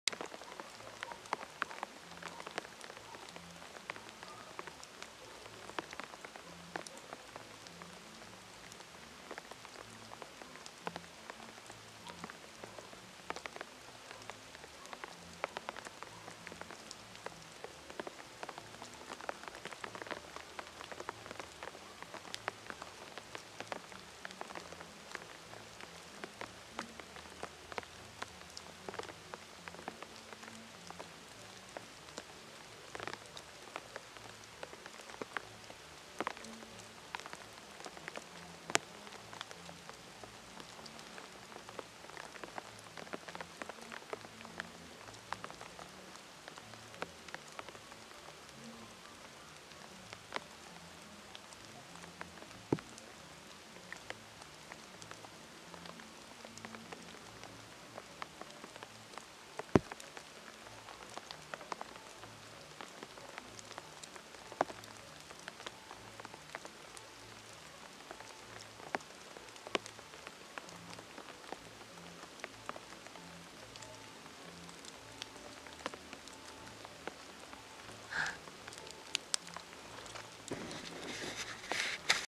Unspotted Saw-whet Owl
lechuza pardo , es un pequeño lechuza que
controla la población de roedores como ratones, tiene una función muy importante en la naturaleza